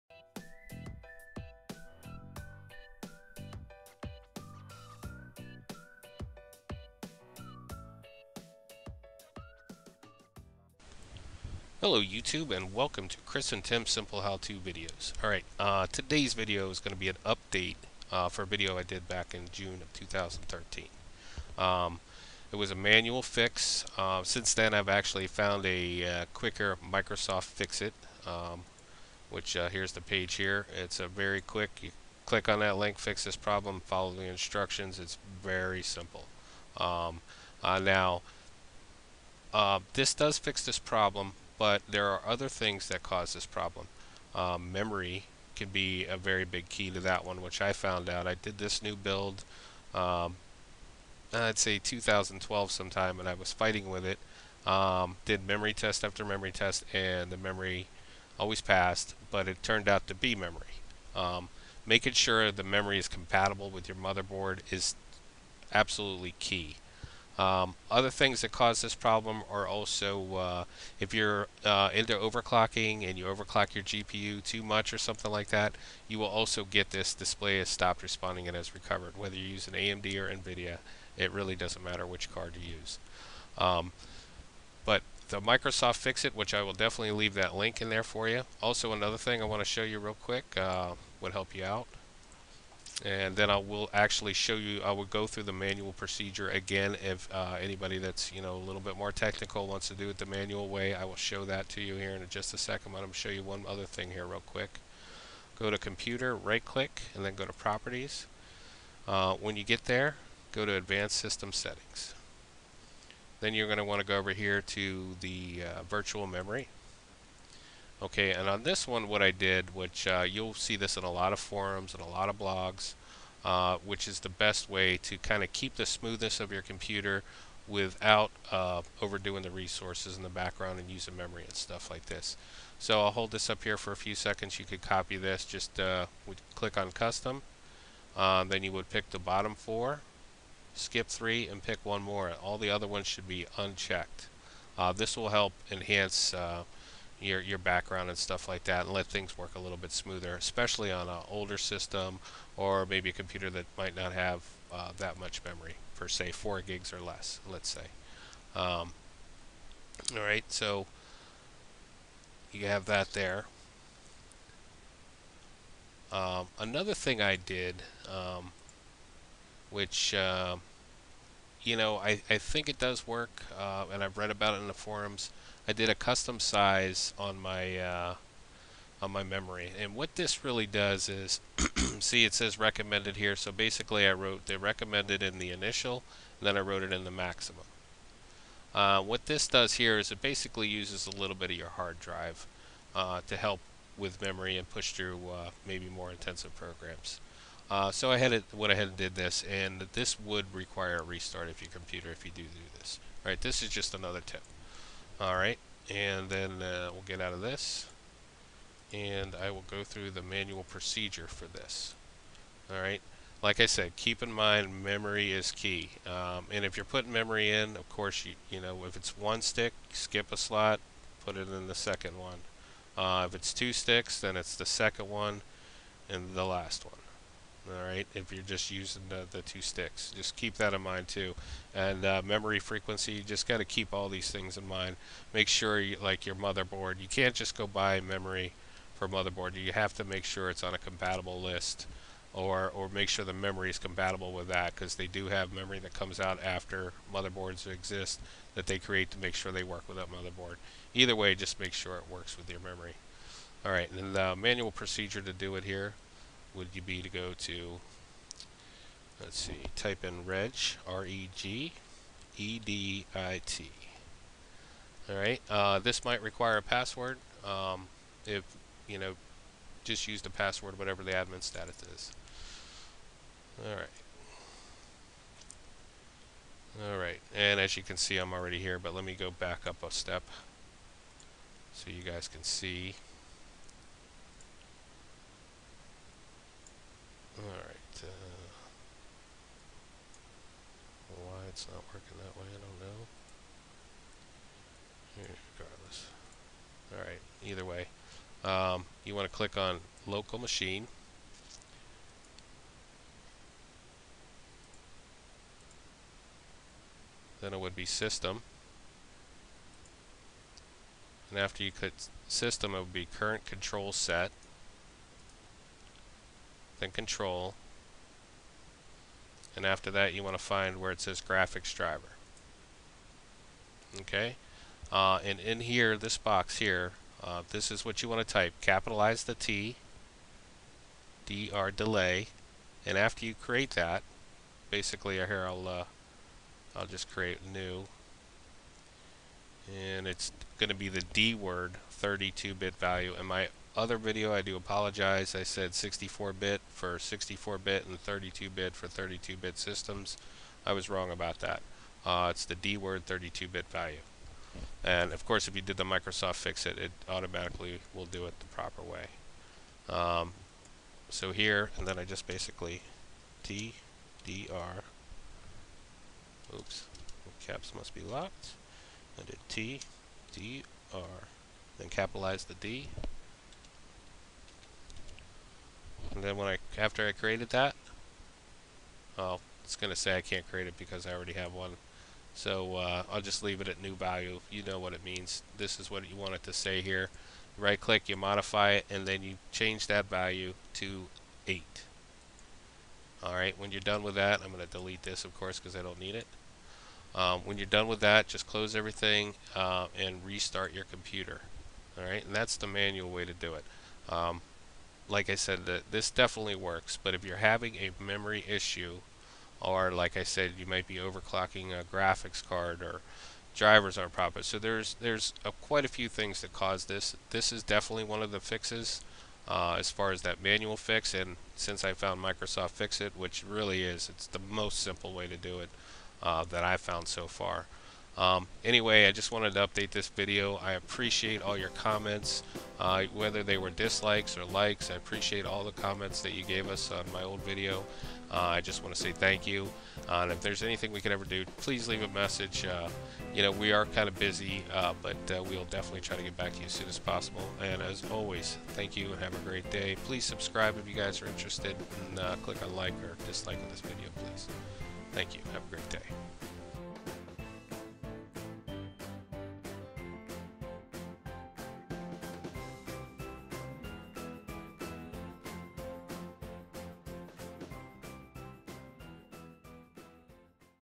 Display Driver Stopped Responding and Has Recovered *UPDATED*
display error fix! For "Display has Stopped Responding And Recovered"32 use only dword ..64 bit system use Qword.. And if your are still getting crashes w/ games or even small browser crashes 98 % sure its memory.. Rule 1 Make sure memory is compatible with your mother board. amd phenom ll processor only read a mem Frequency as high as 1333 keep that in mind! rule 2 dont mix your memory brands. while this dont always cause problems. just not worth the risk. rule 3 make sure memory is in the right slots. your mother board manual will tell you the proper place